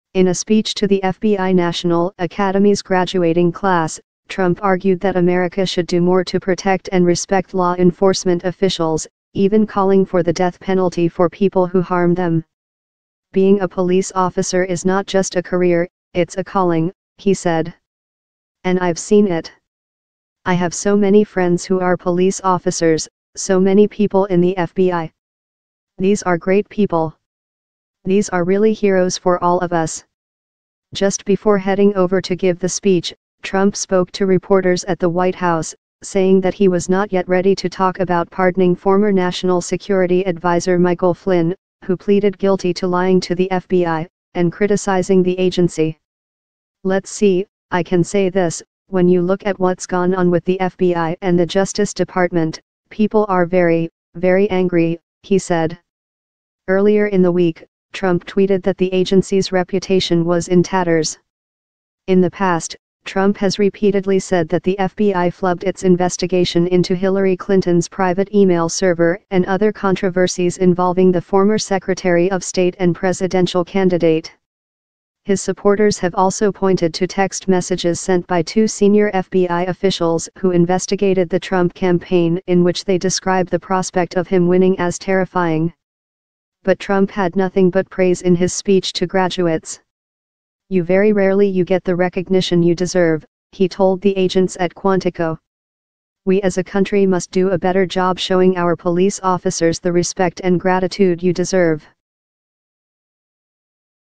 President Trump Bashed The FBI Again. An Hour Later, He Praised Its Agents As Heroes
In a speech to the FBI National Academy graduating class, Trump argued that America should do more to protect and respect law enforcement officials, even calling for the death penalty for people who harm them.